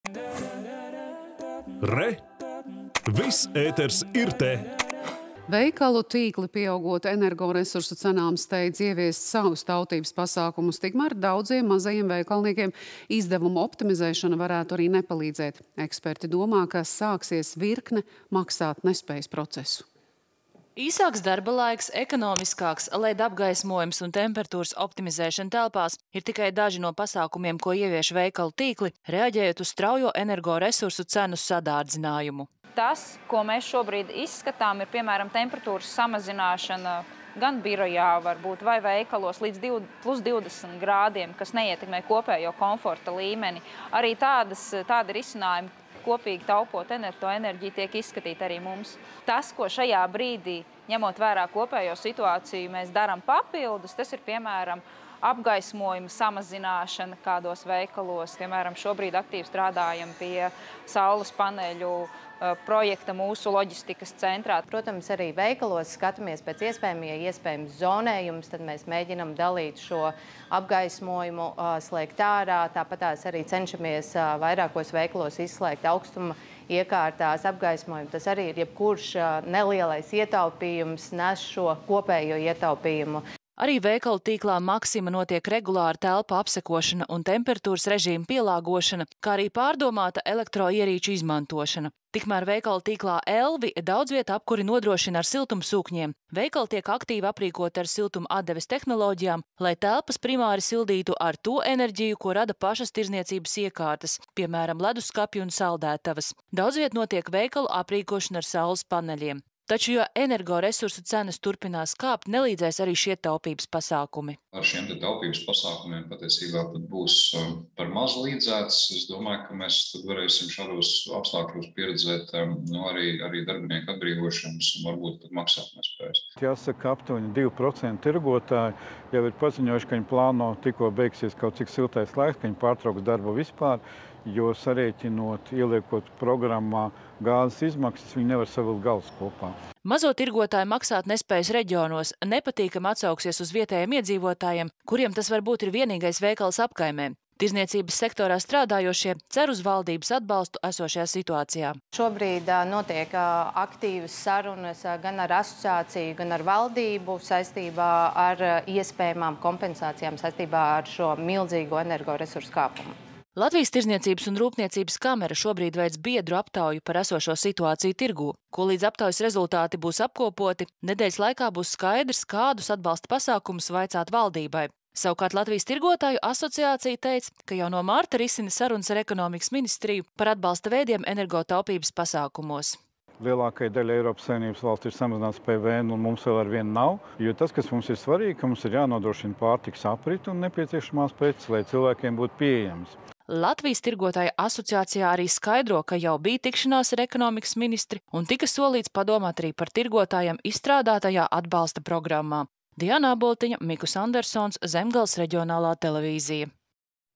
Veikali ievieš taupības pasākumus
Veikalu tīkli, pieaugot energoresursu cenām, steidz ieviest savus taupības pasākumus. Tikmēr daudziem mazajiem veikalniekiem izdevumu optimizēšana varētu arī nepalīdzēt. Eksperti domā, ka sāksies virkne maksātnespējas procesu.